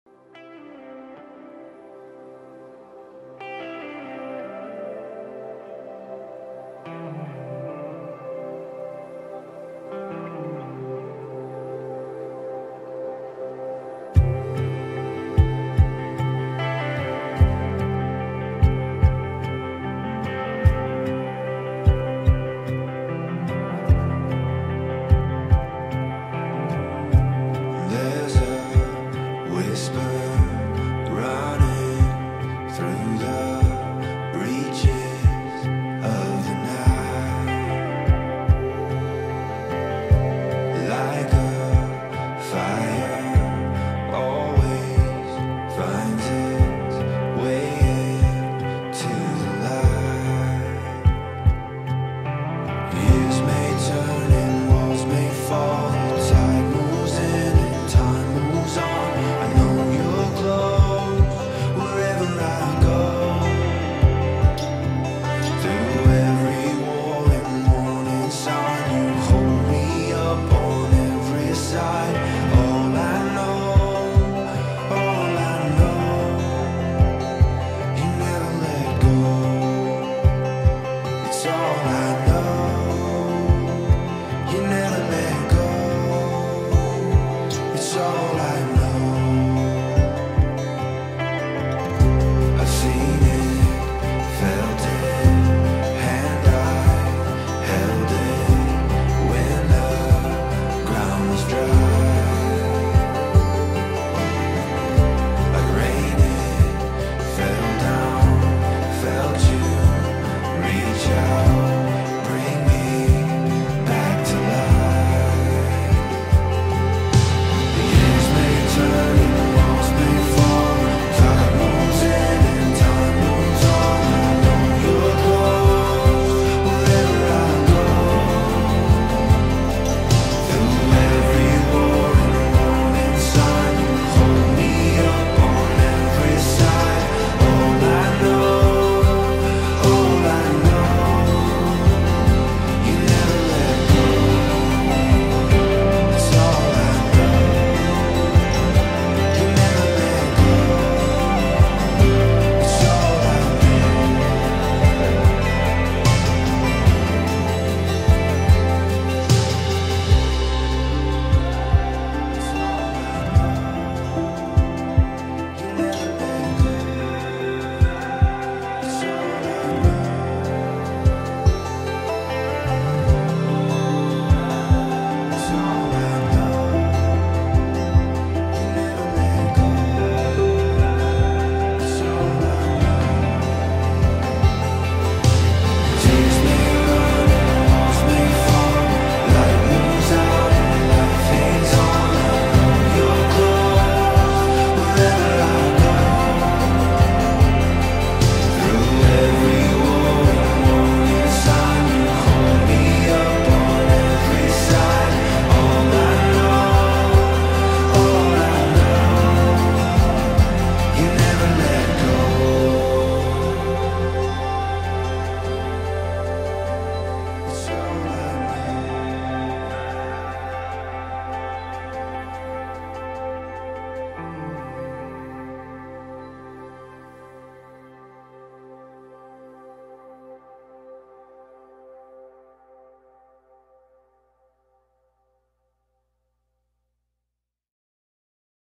EDEN STUDIOS | ASHIK & SNEHA | SAVE THE DATE
a love story in Sydney, Australia | Save the date video for ASHIK & SNEHA

More playlist from our videos

Tamil brahmin wedding : shorturl.at/HIVY7
Hindu wedding : shorturl.at/cioFK